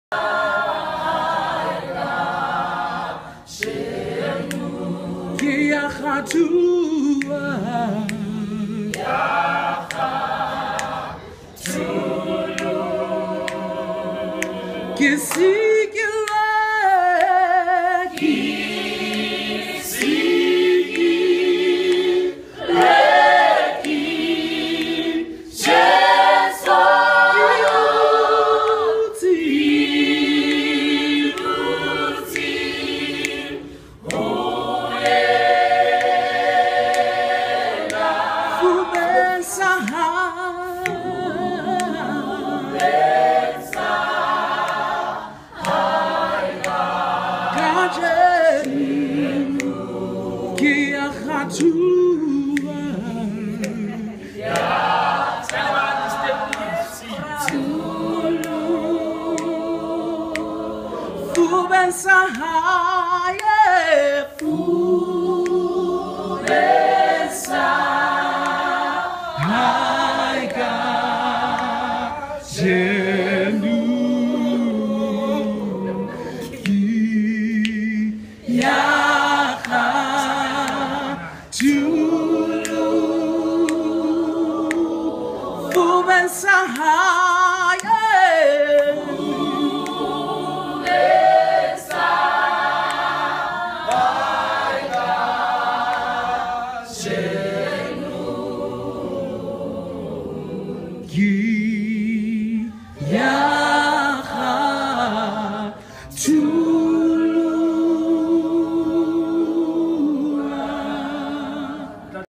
C Sharp Mokeki-Voice Training session in South Africa-Acappella
One of the amazing voices had opportunity to bring together and helped them realize the power in their voices together